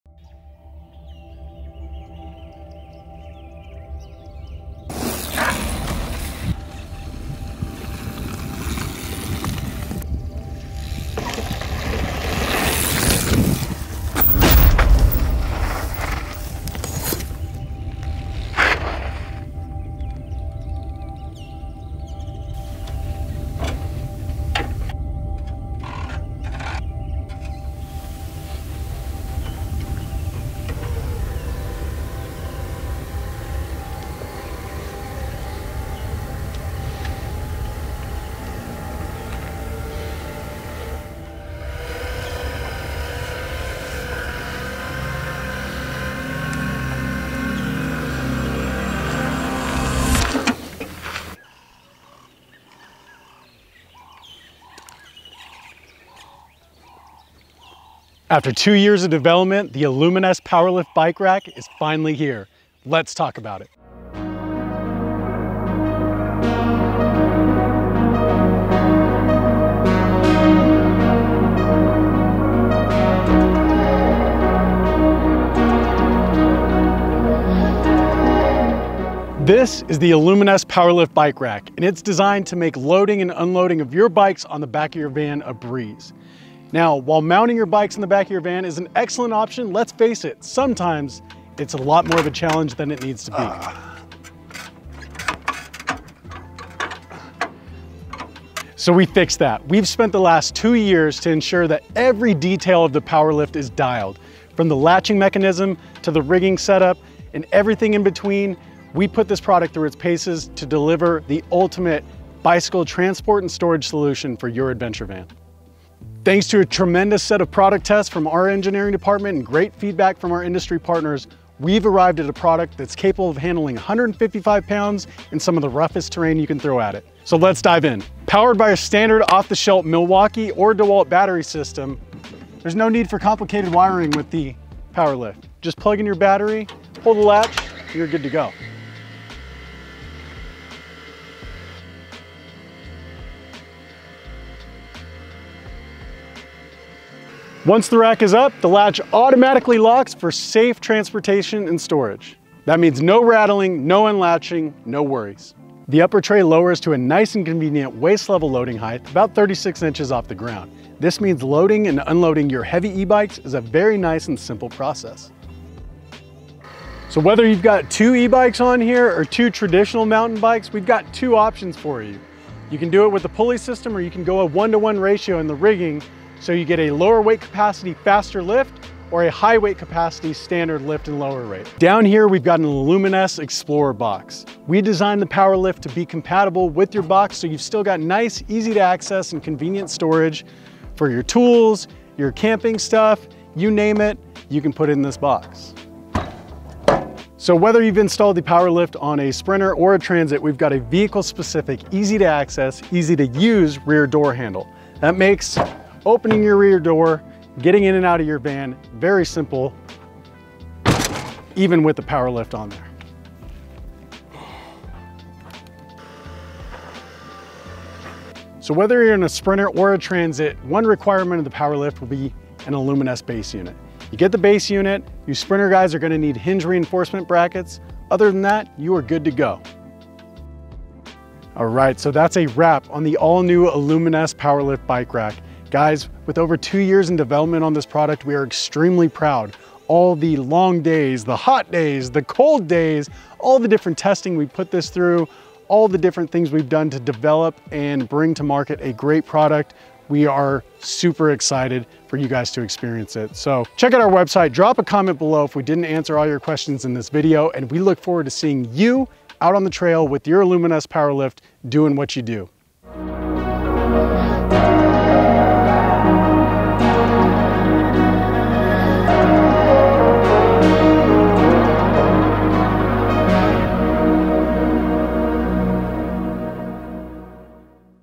The PowerLift Bike Rack - A Van Industry first - Aluminess Products
This is the all-new Aluminess Power Lift Bike Rack, designed to make the hassle of loading and unloading bikes on the back of your van a breeze!

With its innovative lift-assist technology and lightweight aluminum construction, the Aluminess PowerLift can handle two standard e-bikes, or up to 155 lbs capacity. Designed to mount to the Aluminess BackPAC Base Unit , this durable rack has undergone extensive vehicle compliance, heat, and cycle testing to ensure it holds up to your adventures.